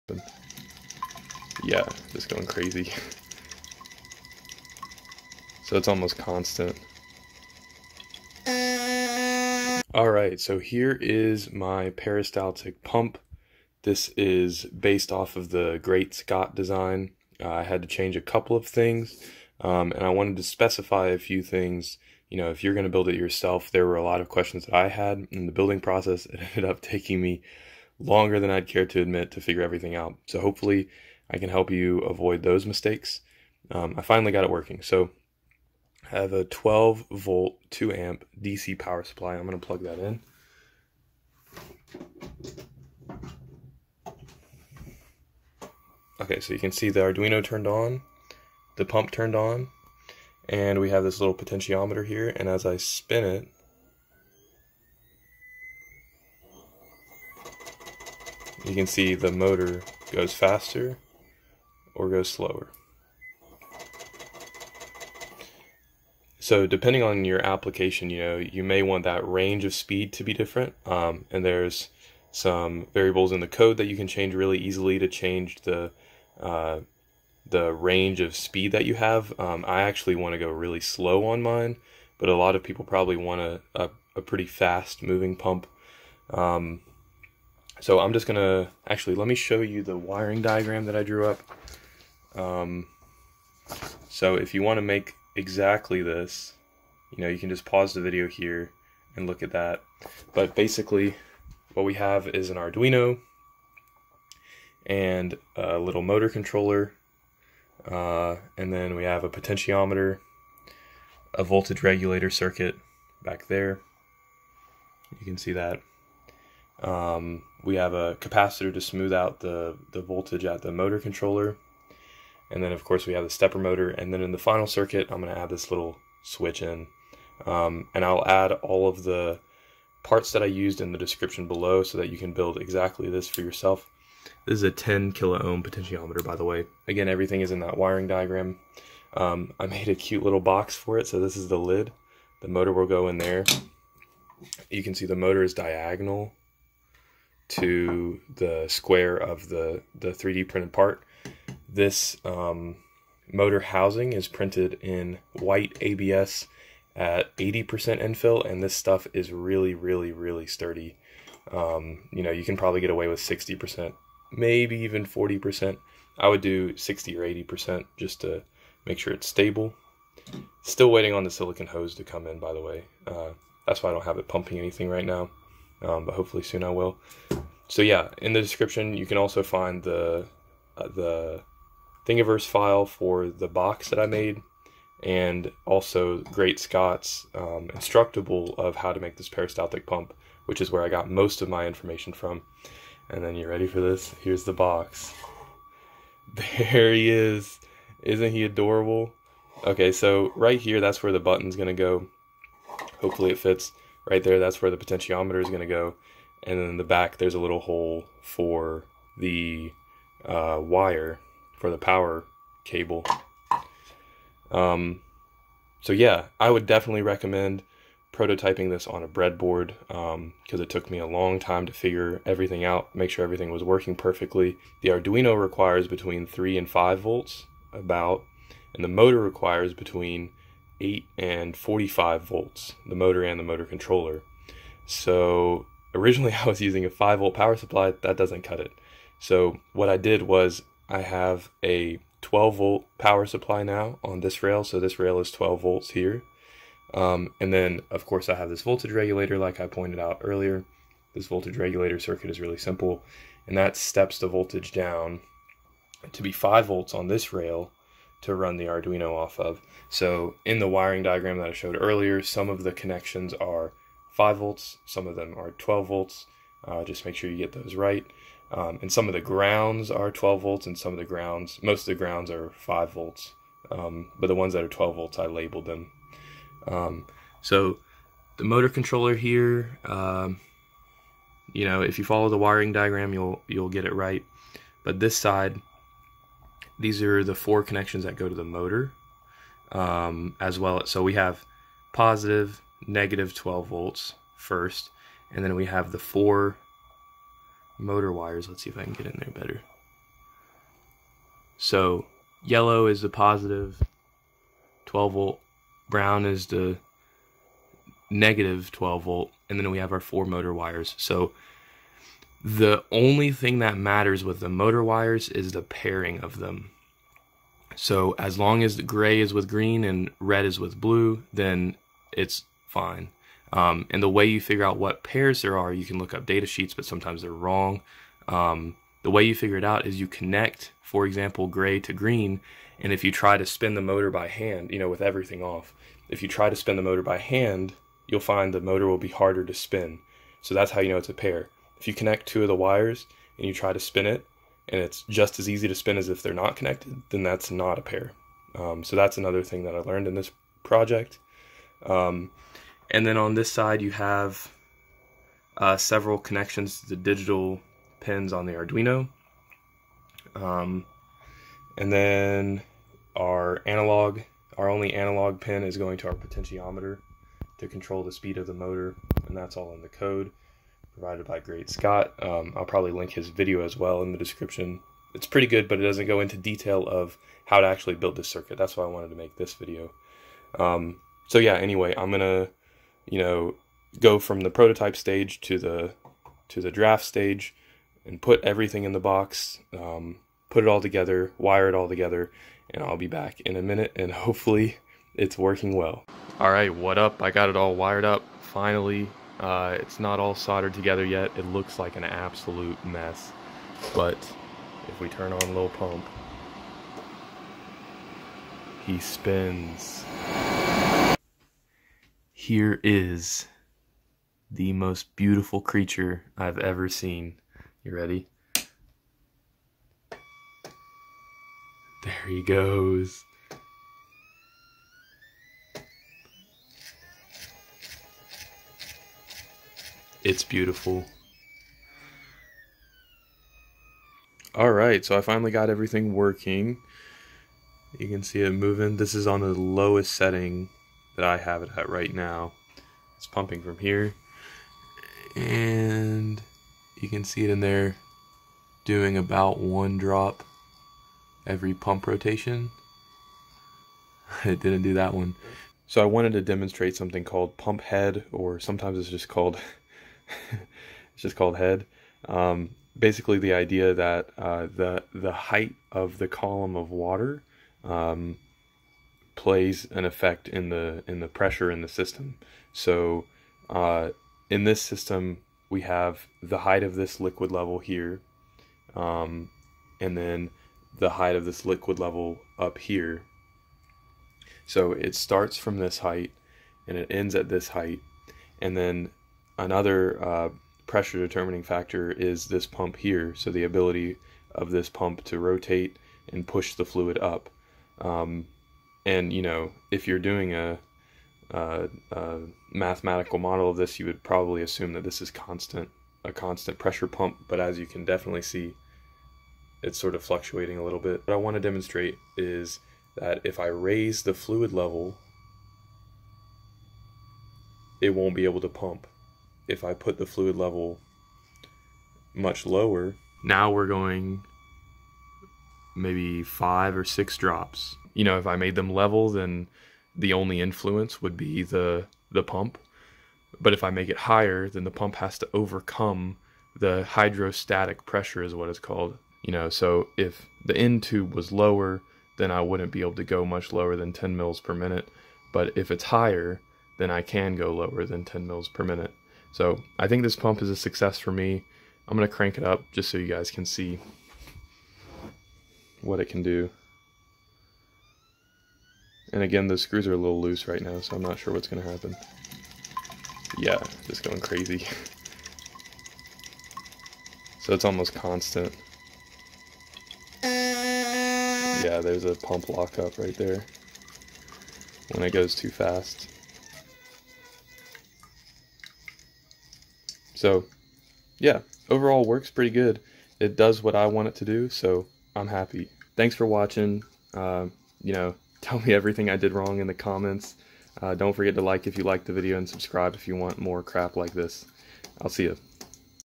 How to Build a Peristaltic Pump (AKA "Lil Pump")
In this video I try to put together a peristaltic pump! This design may be used in later videos to be incorporated into my Tubular Flow Fungal Reactor (TFFR) or my DIY bioreactor.

Please leave a comment letting me know recommendations for future videos!

His channel is amazing! If you haven’t seen him already go check him out: